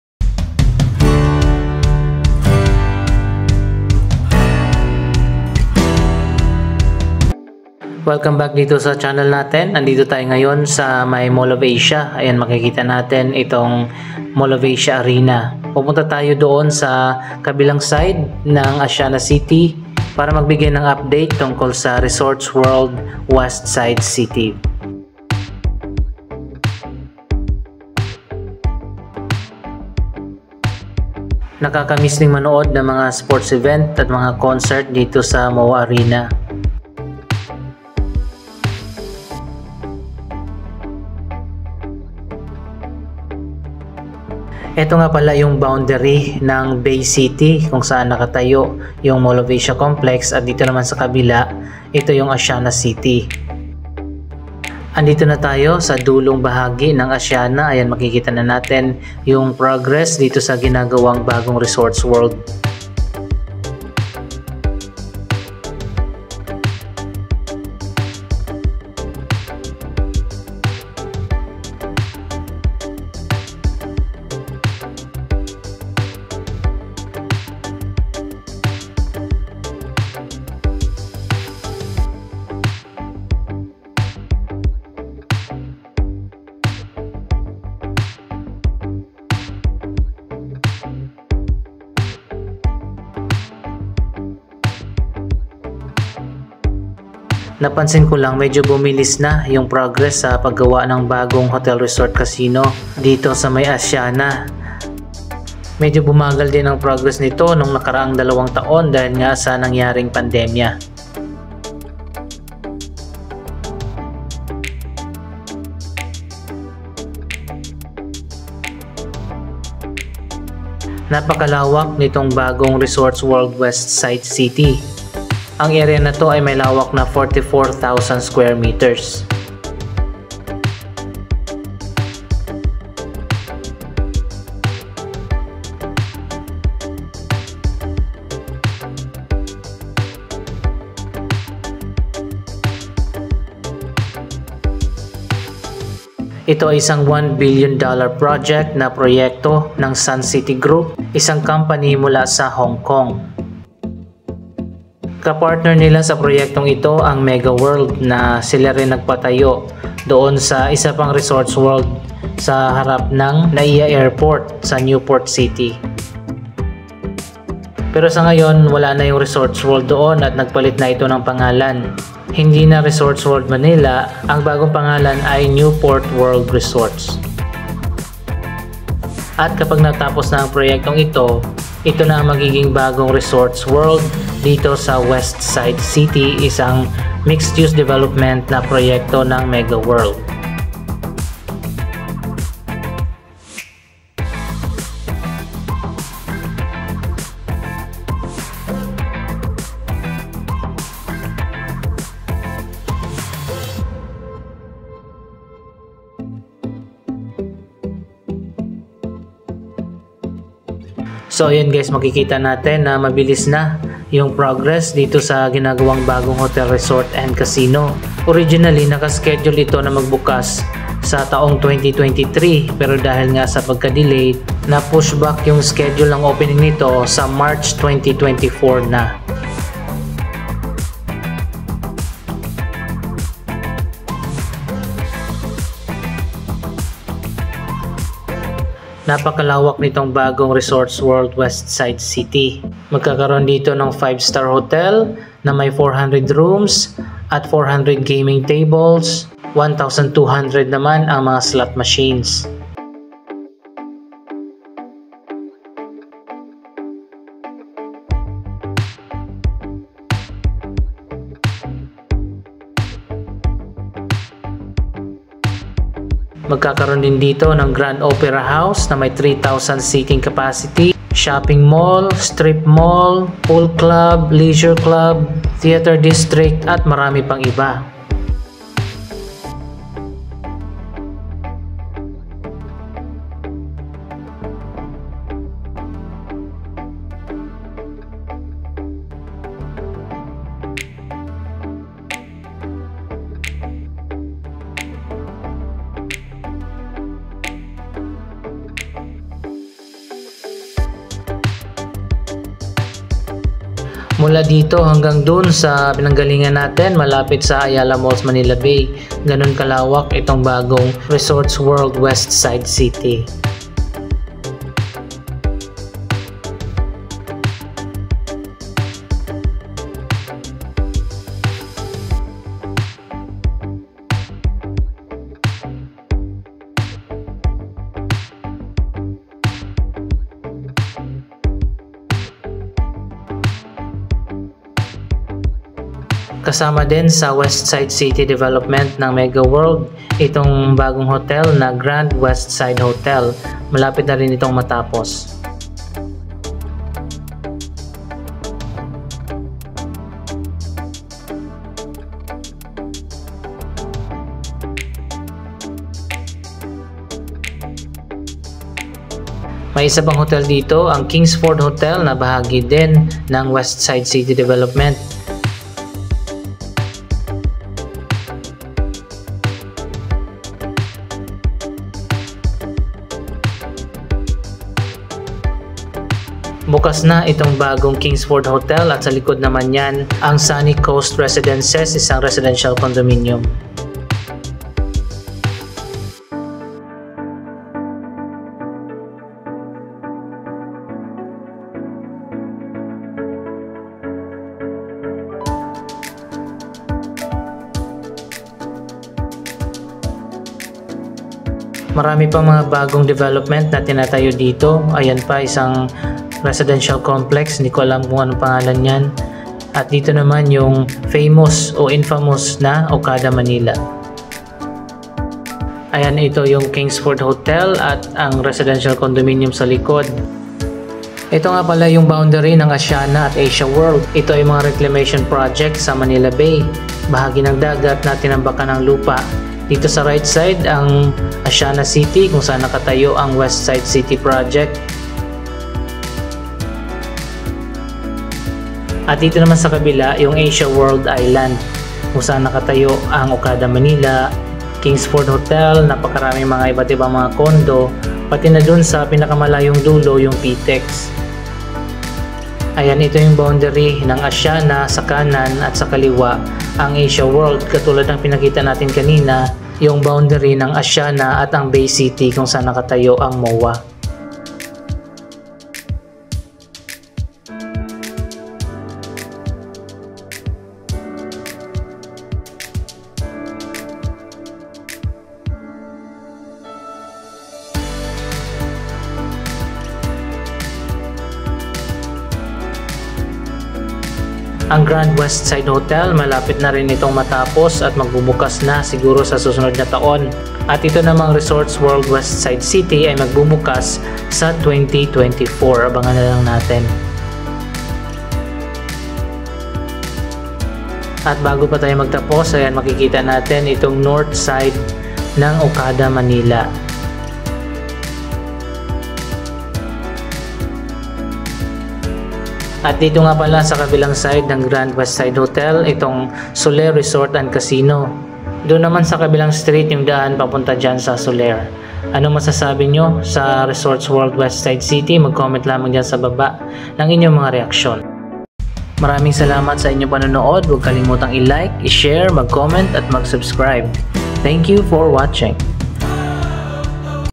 RESORTS WORLD WESTSIDE CITY UPDATE
SunTrust, Suncity Group’s subsidiary in the Philippines, has announced that the Westside City Project’s completion date will be no later than March 31, 2024.

SunTrust has again pushed back the completion date of its new Manila casino. The company previously said it hoped to open the resort by 2023, but has now said the resort will open by March 31, 2024. The reason for the new delay was not explained.

Suntrust signed a joint development agreement with Westside City Resorts World in October 2019, under which it agreed to fund the development and construction costs of the resort and will be appointed as the sole operator and manager of the operation.

The US$1bn project will include 400 gaming tables and 1,200 slot machines, 400 five-star hotel rooms and 960 parking spaces covering 44,000 square metres. Last year, the operator designated local construction giant Megawide as the main contractor for the basement, podium and tower of the main casino hotel.

Suntrust Home Developers has confirmed the timeline for construction of its Westside City project, saying the main hotel now under construction is expected to open in 2024.

Suntrust is 51-percent owned by a local unit of Hong Kong-listed Suncity Holdings.

The Westside City Resorts world project is located in Entertainment City Paranaque,will include a 5-star hotel with at least 400 rooms, a casino with 400 gaming tables and 1,200 slot machines for both mass and VIP markets, and a 960 space parking lot.

It’s the first foray into the Philippines for Hong Kong-listed Suncity, which has been aggressively diversifying its business.

Suncity Group Holdings also retired that the casino hotel portion of the scheme would have circa 400 gaming tables, about 1,200 slot machines, 400 five-star hotel rooms, a pool club and a leisure club, among other attractions.

The final development will be integrated with the shopping malls, theatres, restaurants, and shopping streets, etc. to be built by Westside City Resorts World Inc., which also includes 2,000 additional hotel rooms, a shopping mall, a Grand Opera House with a seating capacity of at least 3000 , restaurants and theatre district, and approximately 2,000 car park spaces.

HIGHER RANKINGS MORE TRAFFIC